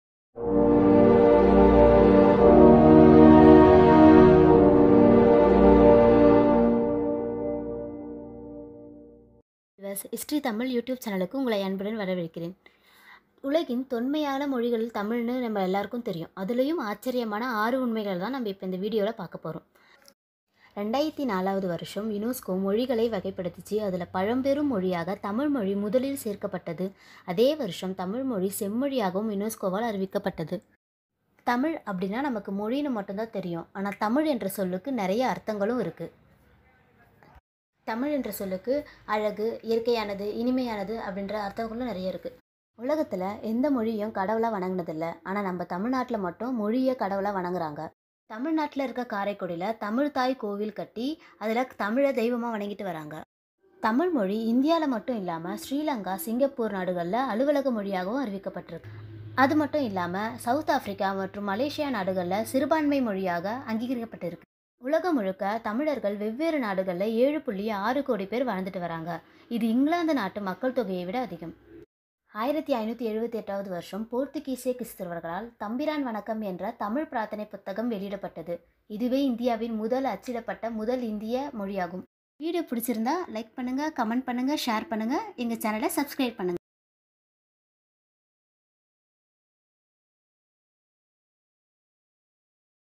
தமிழ் மொழியின் 6 ஆச்சர்யமான உண்மைகள்
Hi All,

We are uploading all about history and vlogs

Some interesting facts in our tamil language

Thank u 😊